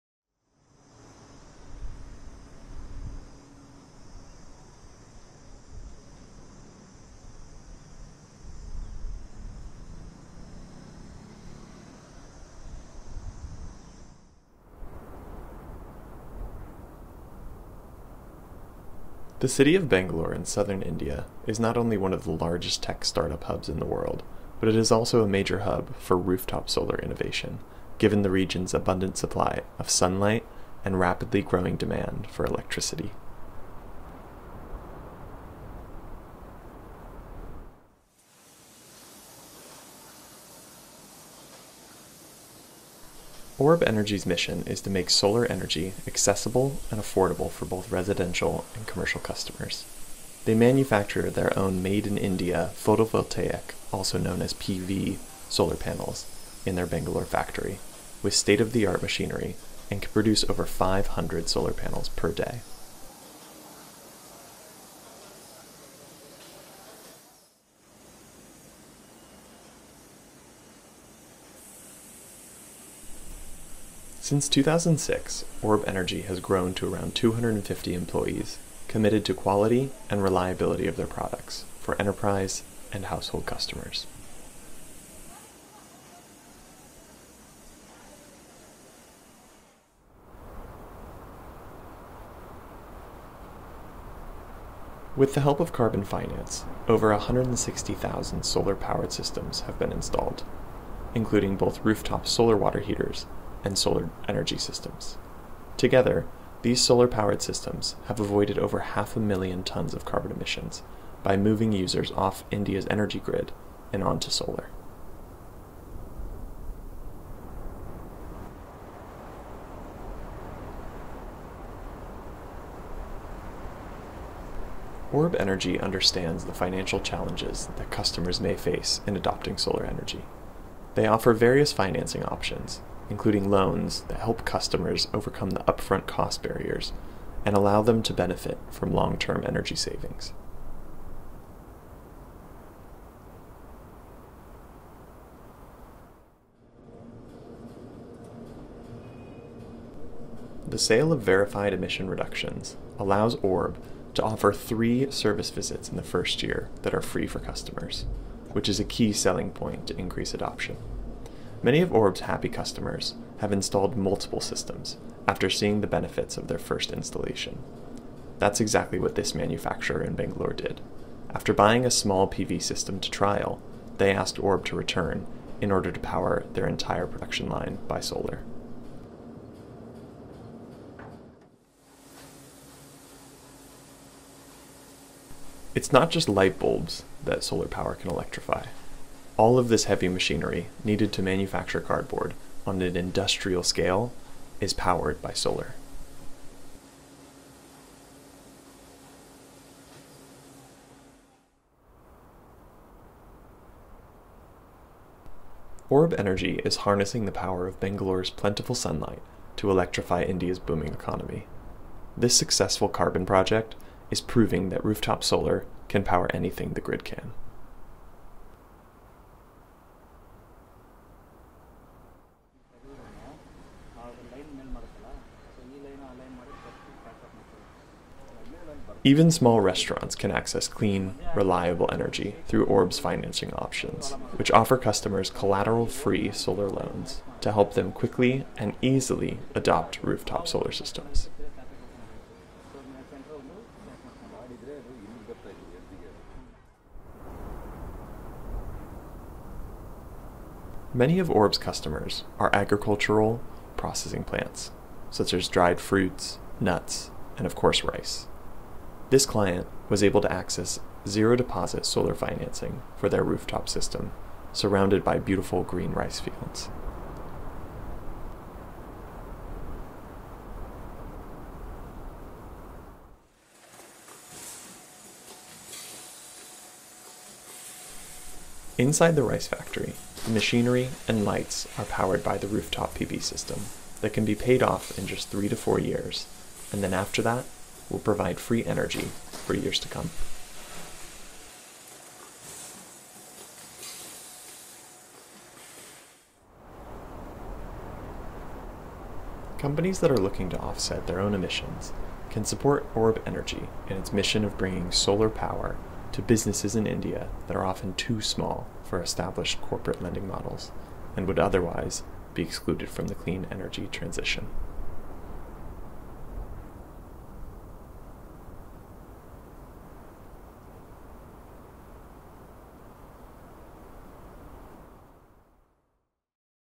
Orb Energy Transition Project in India 360 Video Tour | Climate Impact Partners
Orb Rooftop Solar, India | 360 Video Tour

The city of Bangalore in southern India is not only one of the largest tech start-up hubs in the world, but it is also a major hub for rooftop solar innovation given the region's abundant supply of sunlight and rapidly growing demand for electricity.

Orb Energy's mission is to make solar energy accessible and affordable for both residential and commercial customers. They manufacture their own Made In India photovoltaic, also known as P.V. solar panels in their Bangalore factory with state of the art machinery and can produce over 500 solar panels per day.

Since 2006, Orb Energy has grown to around 250 employees committed to quality and reliability of their products for enterprise and household customers.

With the help of carbon finance, over 160,000 solar-powered systems have been installed, including both rooftop solar water heaters and solar energy systems. Together, these solar-powered systems have avoided over half a million tonnes of carbon emissions by moving users off India's energy grid and on to solar.

Orb Energy understands the financial challenges that customers may face in adopting solar energy. They offer various financing options, including loans that help customers overcome the upfront cost barrier and allow them to benefit from long-term energy savings.

The sale of verified emission reductions allows Orb to offer three service visits in the first year that are free for customers, which is a key selling point to increase adoption. Many of Orb's happy customers have installed multiple systems after seeing the benefits of their first installation. That's exactly what this manufacturer in Bangalore did, after buying a small PV system to trial, they asked Orb to return in order to power their entire production line by solar.

It's not just lightbulbs that solar power can electrify, all of this heavy machinery needed to manufacture cardboard on an industrial scale is powered by solar.

Orb Energy is harnessing the power of Bangalore's plentiful sunlight to electrify India's booming economy. This successful carbon project is proving that rooftop solar can power anything the grid can.

Even small restaurants can access clean, reliable energy through Orb's financing options which offer customers collateral-free solar loans to help them quickly and easily adopt rooftop solar systems.

Many of Orb's customers are agricultural processing plants such as dried fruits, nuts, and of course rice. This client was able to access zero deposit solar financing for their rooftop system surrounded by beautiful green rice fields.

Inside the rice factory, the machinery and lights are powered by the rooftop PV system that can be paid off in three to four years and then after that, will provide free energy for years to come.

Companies that are looking to offset their own emissions can support Orb Energy in its mission of bringing solar power to businesses in India that are often too small for established corporate lending models and would otherwise be excluded from the clean energy transition.